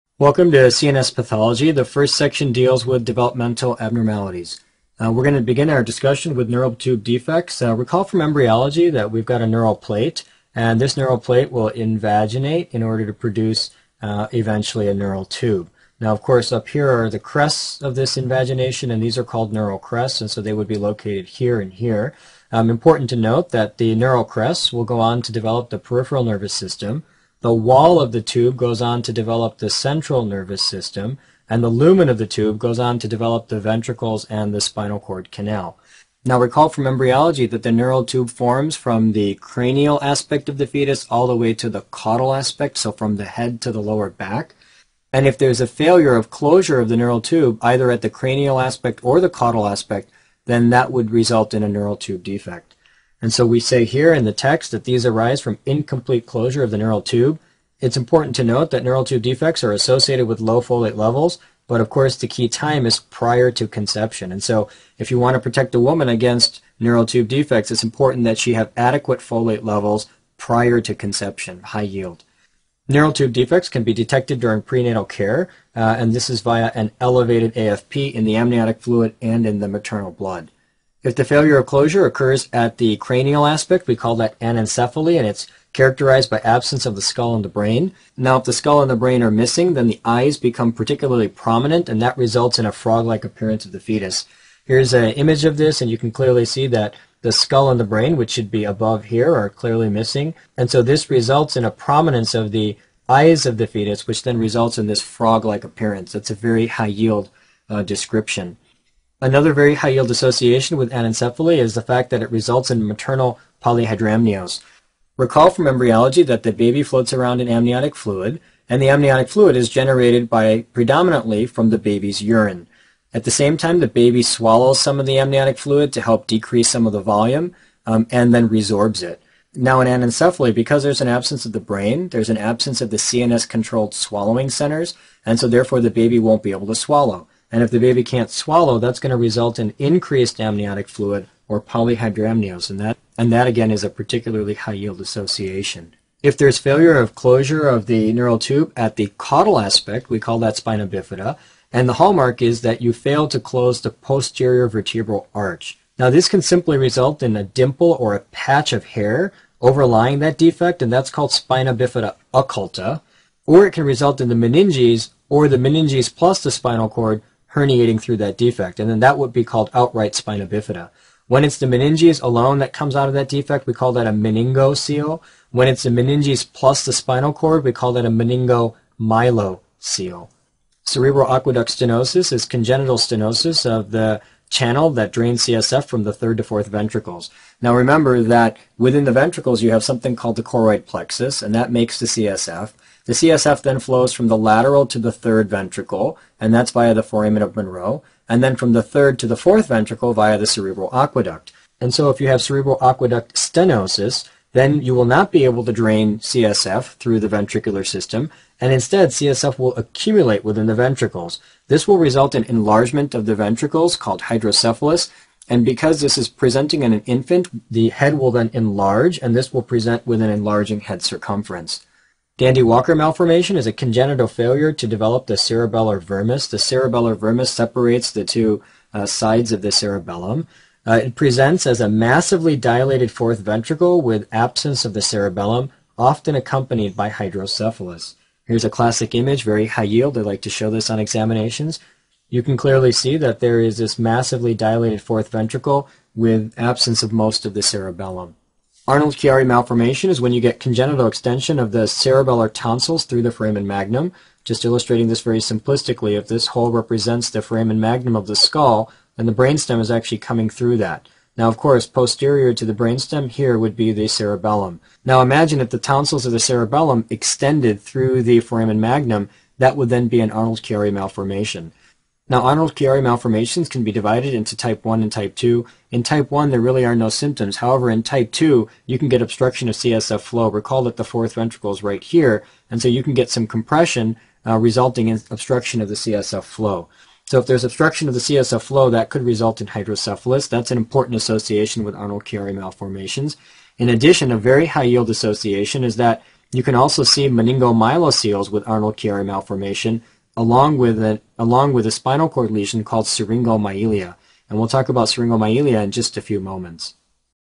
Pathology of Developmental Anomalies of CNS Pathoma Lectures word by word explained
Pathology lectures by Pathoma explained word by word in very easy way in the form of slides. This will help you understand pathology of Developmental Anomalies of CNS easily.